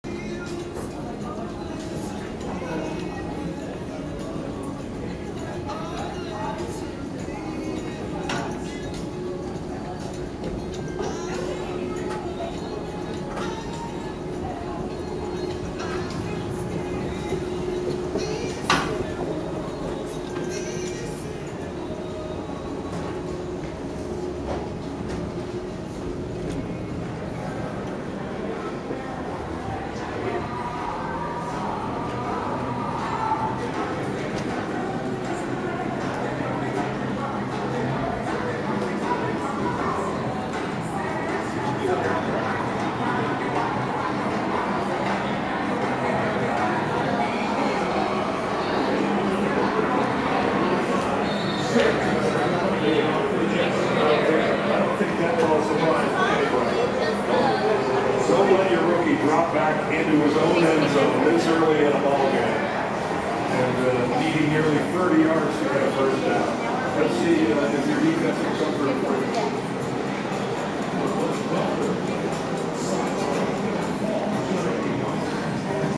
Walk Fom Signature Tower 3 to MGM Grand part 2
Walking from our room to the MGM grand studio walkway from Tower 3 at the Signature, part 2